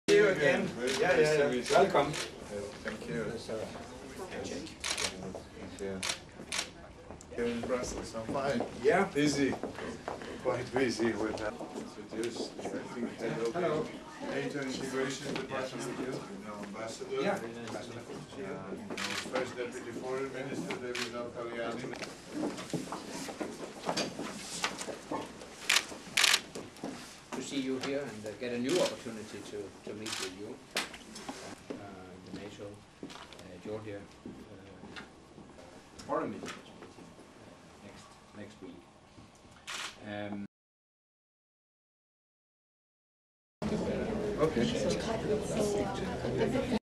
NATO-Georgia Commission (NGC) meeting in Brussels
The State Minister of Georgia on European and Euro-Atlantic Integration Alex Petriashvili in Brussels participated in NATO-GEORGIA (NGC) Commission meeting. At the NGC meeting Alex Petriashvili informed the NATO ambassadors about the democratic progress shown in Georgia, in particular, reforms implemented under the Annual National Program (ANP) 2013.

Within the framework of the visit, the State Minister conducted bilateral meeting with NATO Secretary General Anders Fogh Rasmussen.